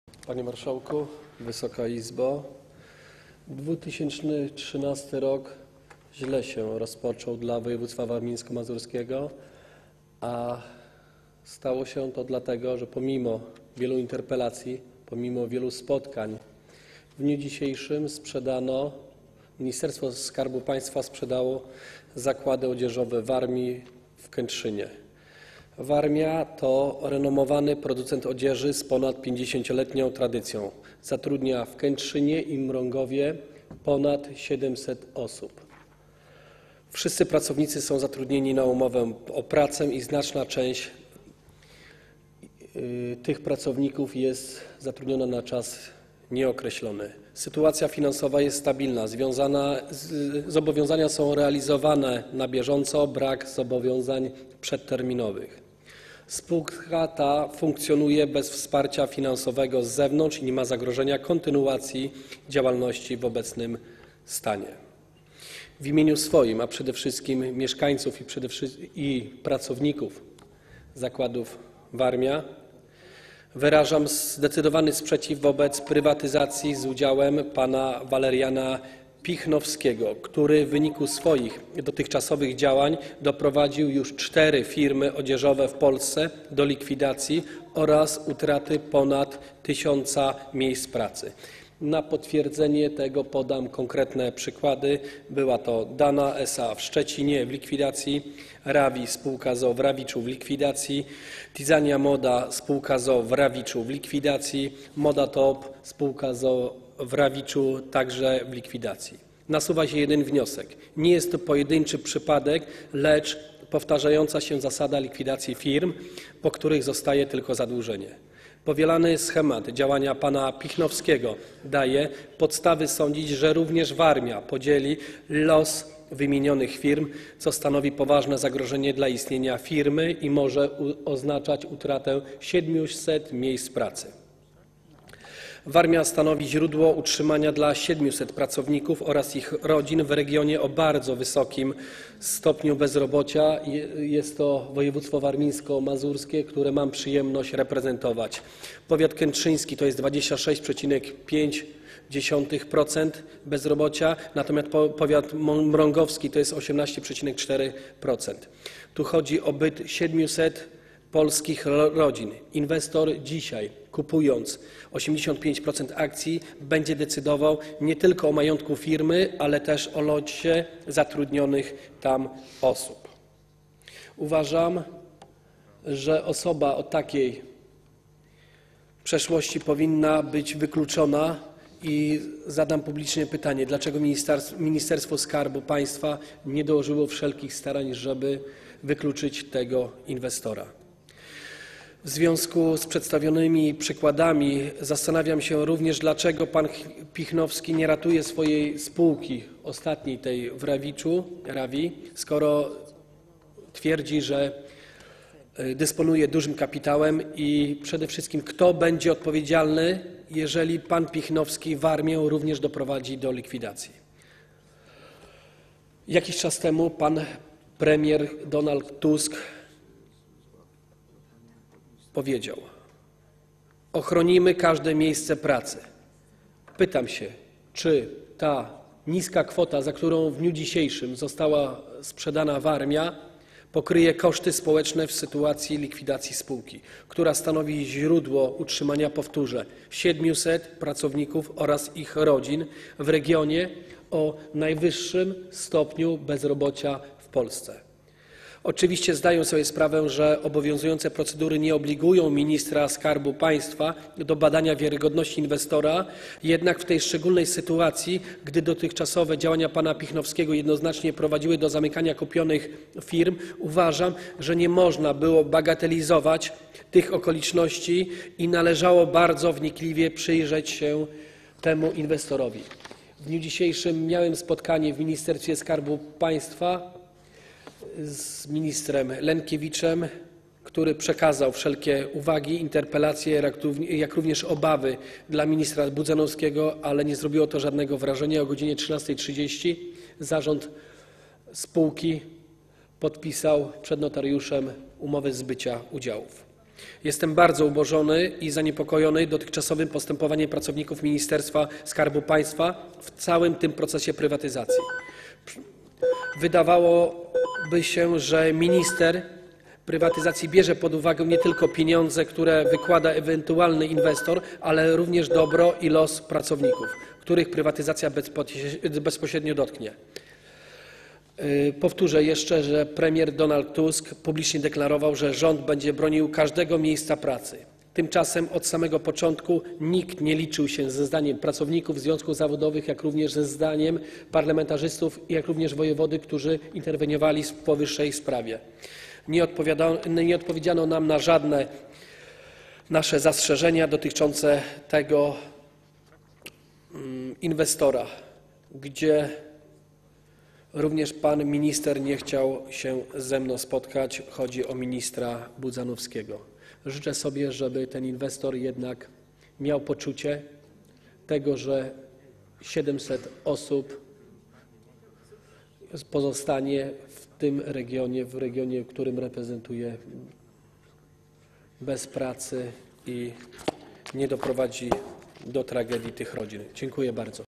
[383/396] Wojciech Penkalski: Rok 2013 źle się rozpoczął dla województwa warmińsko-mazurski...
Posiedzenie Sejmu nr 31
3 stycznia 2013 r. 22:53:44
Poseł Wojciech Penkalski:

Panie Marszałku! Wysoka Izbo! Rok 2013 źle się rozpoczął dla województwa warmińsko-mazurskiego, a stało się tak dlatego, że pomimo wielu interpelacji, pomimo wielu spotkań w dniu dzisiejszym Ministerstwo Skarbu Państwa sprzedało zakłady odzieżowe Warmia w Kętrzynie.

Warmia to renomowany producent odzieży z ponadpięćdziesięcioletnią tradycją. Zatrudnia w Kętrzynie i Mrągowie ponad 700 osób. Wszyscy pracownicy są zatrudnieni na umowę o pracę, znaczna część tych pracowników jest zatrudniona na czas nieokreślony. Sytuacja finansowa jest stabilna, zobowiązania są realizowane na bieżąco, brak zobowiązań przeterminowanych. Spółka ta funkcjonuje bez wsparcia finansowego z zewnątrz i nie ma zagrożenia kontynuacji działalności w obecnym stanie.

W imieniu swoim oraz przede wszystkim mieszkańców i pracowników zakładów Warmia wyrażam zdecydowany sprzeciw wobec prywatyzacji z udziałem pana Waleriana Pichnowskiego, który w wyniku swoich dotychczasowych działań doprowadził już cztery firmy odzieżowe w Polsce do likwidacji oraz utraty ponad tysiąca miejsc pracy.

Na potwierdzenie tego podam konkretne przykłady. Były to: Dana SA w Szczecinie -- w likwidacji, Rawi sp. z o.o. w Rawiczu -- w likwidacji, Tiziana Moda sp. z o.o. w Rawiczu -- w likwidacji, Moda Top sp. z o.o. w Rawiczu -- także w likwidacji.

Nasuwa się jeden wniosek. Nie jest to pojedynczy przypadek, lecz powtarzająca się zasada likwidacji firm, po których zostaje tylko zadłużenie. Powielany schemat działania pana Pichnowskiego daje podstawy sądzić, że również Warmia podzieli los wymienionych firm, co stanowi poważne zagrożenie dla istnienia firmy i może oznaczać utratę 700 miejsc pracy.

Warmia stanowi źródło utrzymania dla 700 pracowników oraz ich rodzin w regionie o bardzo wysokim stopniu bezrobocia. Jest to województwo warmińsko-mazurskie, które mam przyjemność reprezentować. Powiat kętrzyński to jest poziom bezrobocia wynoszący 26,5%, natomiast powiat mrągowski -- 18,4%. Tu chodzi o byt 700 polskich rodzin. Inwestor, kupując 85% akcji, będzie decydował nie tylko o majątku firmy, lecz także o losie zatrudnionych tam osób. Uważam, że osoba o takiej przeszłości powinna być wykluczona. I zadam publicznie pytanie: Dlaczego Ministerstwo Skarbu Państwa nie dołożyło wszelkich starań, żeby wykluczyć tego inwestora?

W związku z przedstawionymi przykładami zastanawiam się również, dlaczego pan Pichnowski nie ratuje swojej spółki, tej ostatniej w Rawiczu, Rawi, skoro twierdzi, że dysponuje dużym kapitałem. I przede wszystkim kto będzie odpowiedzialny, jeżeli pan Pichnowski również Warmię doprowadzi do likwidacji?

Jakiś czas temu pan premier Donald Tusk powiedział: ochronimy każde miejsce pracy. Pytam się, czy ta niska kwota, za którą w dniu dzisiejszym została sprzedana Warmia, pokryje koszty społeczne w sytuacji likwidacji spółki, która stanowi źródło utrzymania -- powtórzę -- 700 pracowników oraz ich rodzin w regionie o najwyższej stopnie bezrobocia w Polsce.

Oczywiście zdaję sobie sprawę, że obowiązujące procedury nie obligują ministra skarbu państwa do badania wiarygodności inwestora, jednak w tej szczególnej sytuacji, gdy dotychczasowe działania pana Pichnowskiego jednoznacznie prowadziły do zamykania kupionych firm, uważam, że nie można było bagatelizować tych okoliczności i należało bardzo wnikliwie przyjrzeć się temu inwestorowi.

W dniu dzisiejszym miałem spotkanie w Ministerstwie Skarbu Państwa z ministrem Lenkiewiczem, który przekazał wszelkie uwagi, interpelacje, jak również obawy ministrowi Budzanowskiemu, ale nie zrobiło to żadnego wrażenia. O godz. 13.30 zarząd spółki podpisał przed notariuszem umowy zbycia udziałów.

Jestem bardzo oburzony i zaniepokojony dotychczasowym postępowaniem pracowników Ministerstwa Skarbu Państwa w całym tym procesie prywatyzacji.

(Dzwonek)

Wydawałoby...